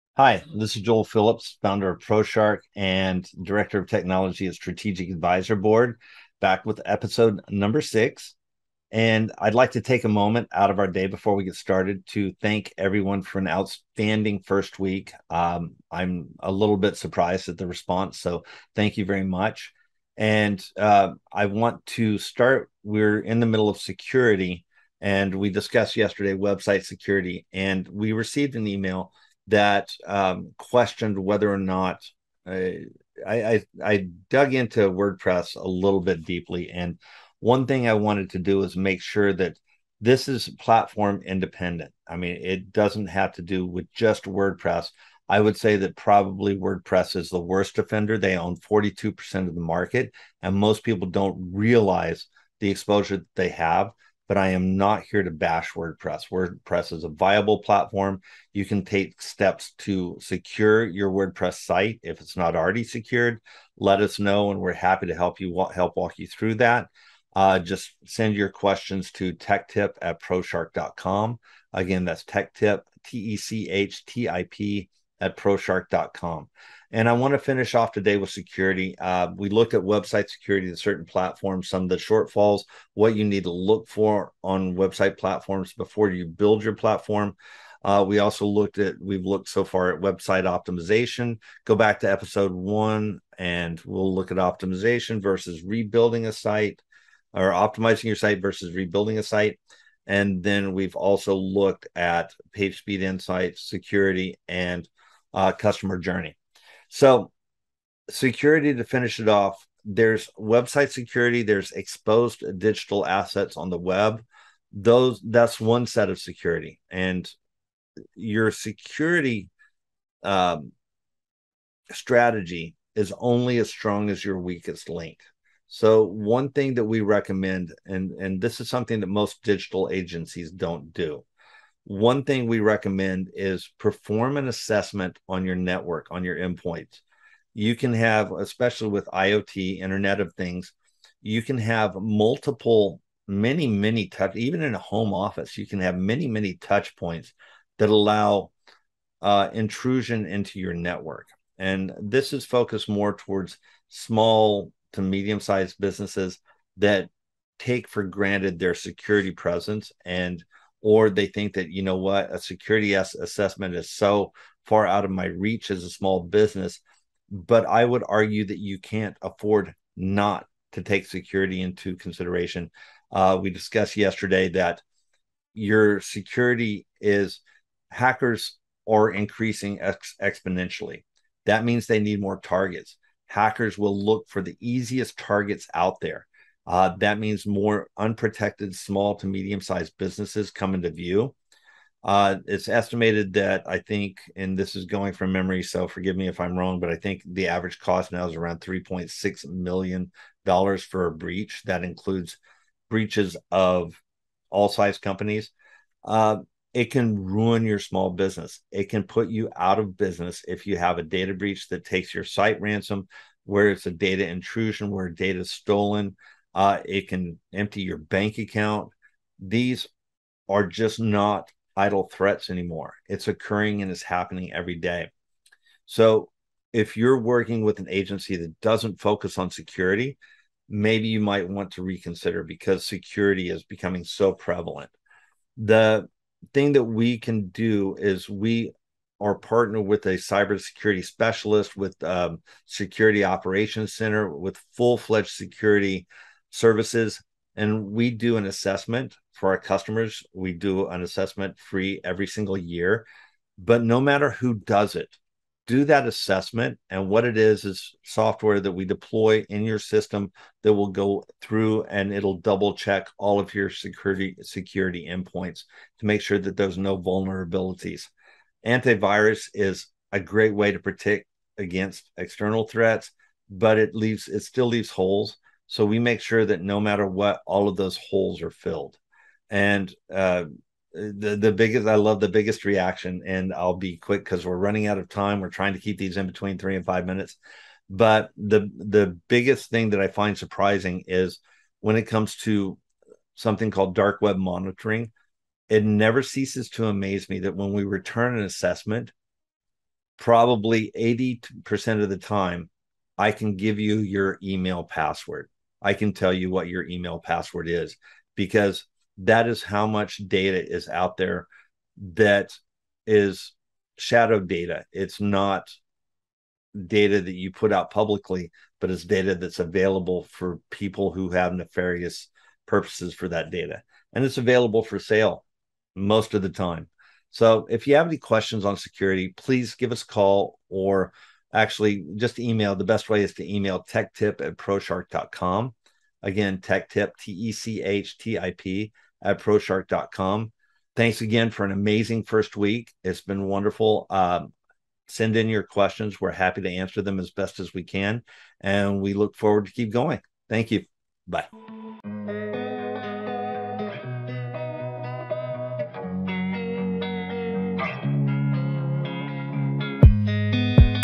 Website Security Part 2 - Network Security - The Ultimate Tech Talk with Joel Phillips | Video 6
ownload the International Best Selling Book Today to Grow Past 7 Figures

Wrapping up the Security episodes, Proshark CEO / Founder and Strategic Advisor Board Director of Technology, covers network security in both physical and remote environments. Acknowledging that there is a great deal more information than can be covered in this format, Joel encourages viewers to reach out to security experts to learn more. If you would like to ask questions pertaining to security or questions about other topics, simply email and we will get them answered for you. Points discussed include running an assessment on your network whether it is in office, remote at home or some hybrid of the two at least once a year. Small to medium-sized business thinks security is out of reach for them, but it isn't. In fact, security is reasonable and business owners cannot afford NOT to implement security measures.

Some of your information is probably already on the dark web.

International best-selling book Be Good, Be Brief, Be Gone—available for free